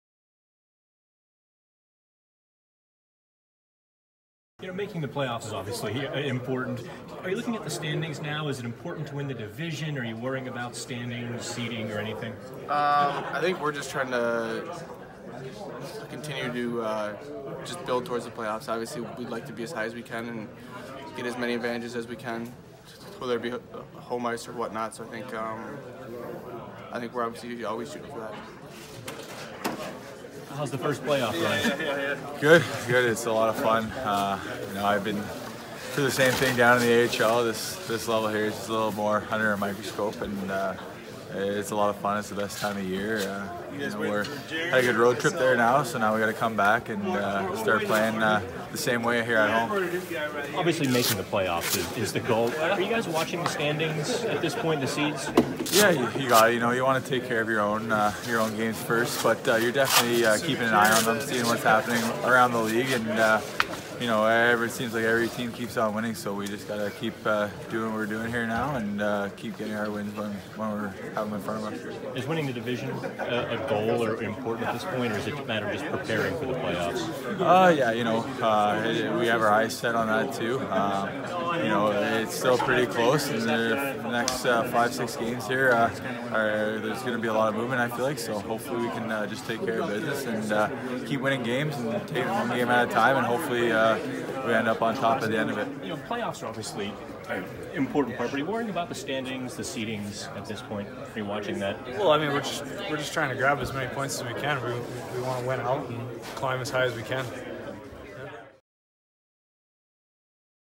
Penguins Locker Room: Are Playoff Seedings Important? | PHN
The penguins respond to the question of whether or not playoff seedings are important.